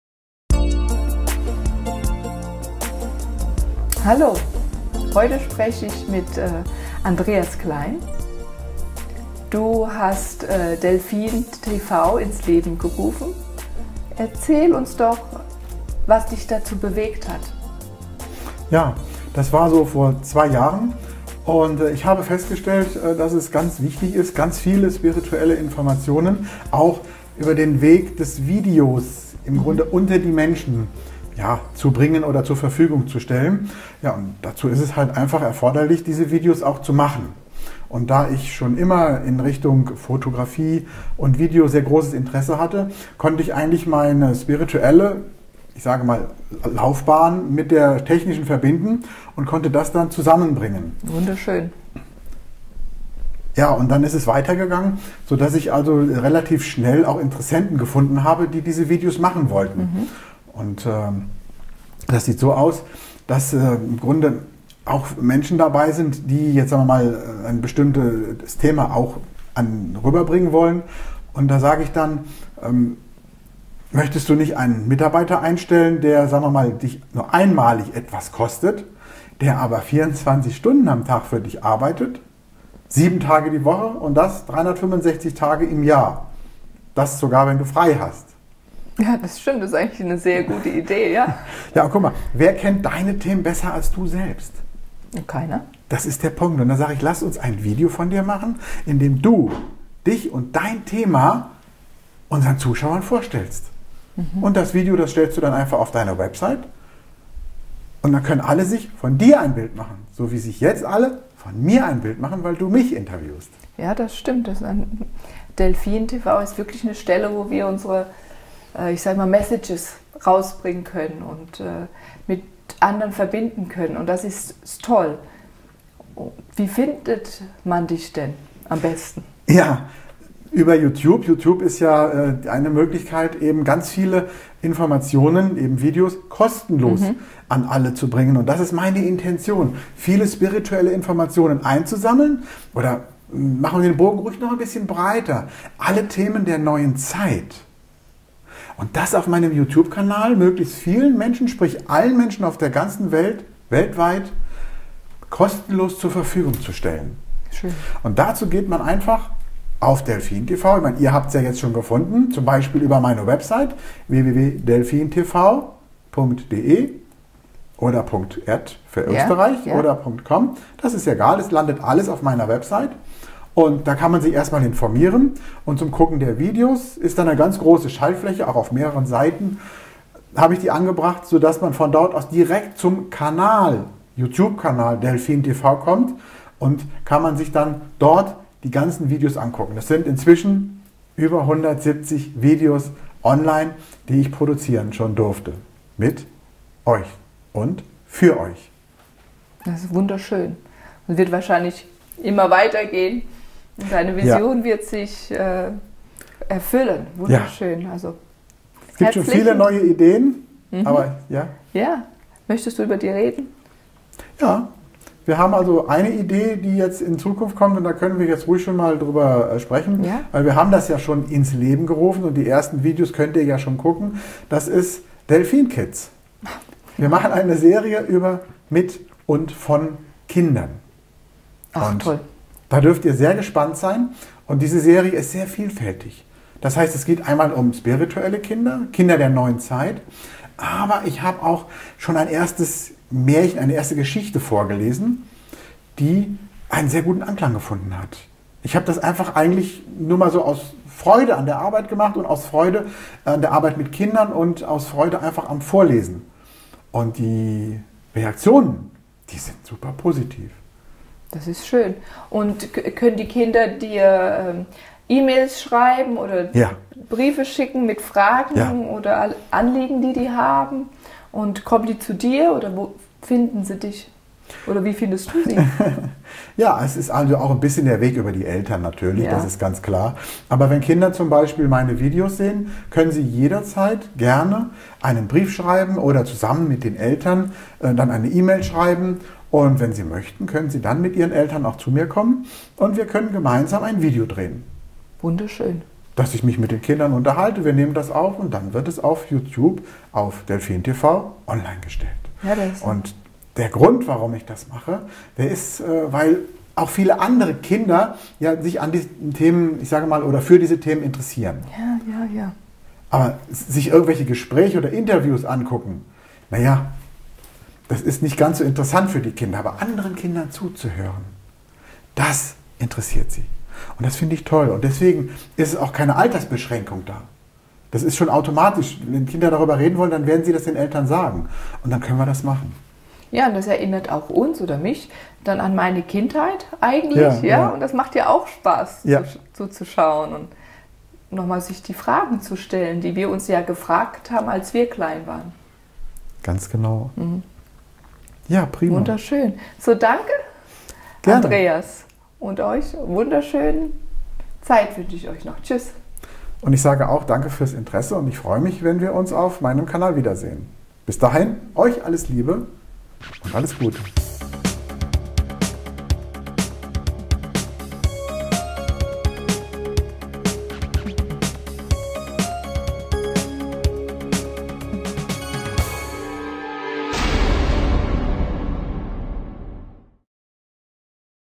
R.-Andreas Klein über DelphinTV 2
- DelphinTV ... R.-Andreas Klein ... Vorstellung ... Image
Motivation, Intention, Idee und Zukünftige Projekte von DelphinTV

Video-Aufnahme:
R.-Andreas Klein